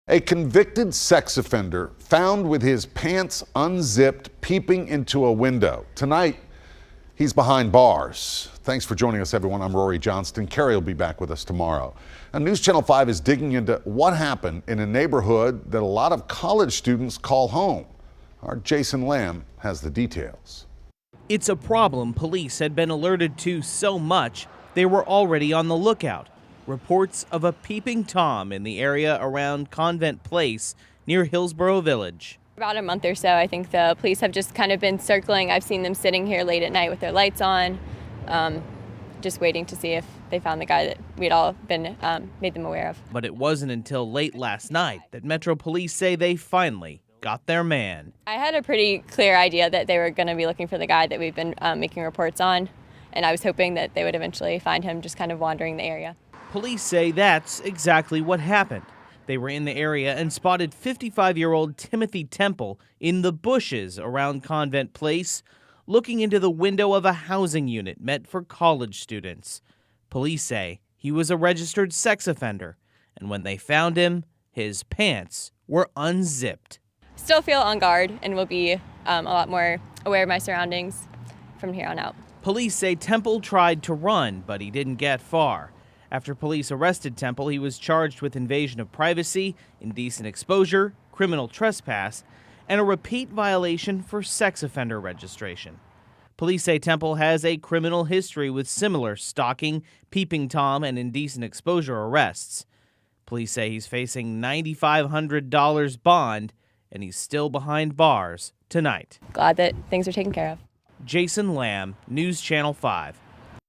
Peeping tom suspect caught, arrested on multiple charges
Registered sex offender Timothy Temple, 55, was arrested by Midtown Hills officers late Sunday night for invasion of privacy/peeping tom and indecent exposure, among other related charges.